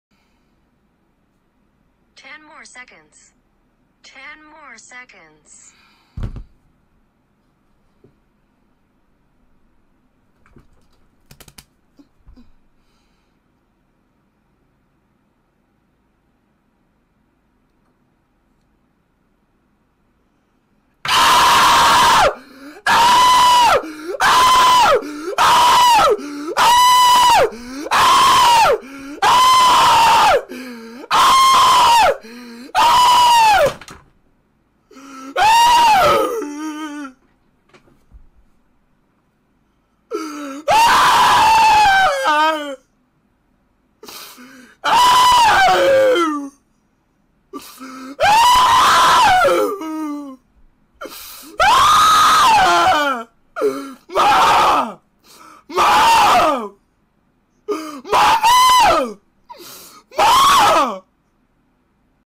ishowspeed try kylie jenner challenge and goes wrong for him
kids don't reproduce that at home